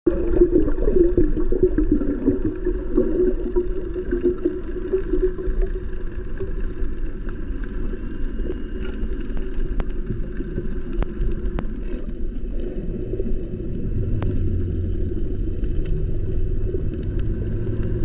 Beautiful triggerfish in Makadi Bay, Hurghada
October 2022 in Makadi Bay, Hurghada i filmed this adorable triggerfish doing their business.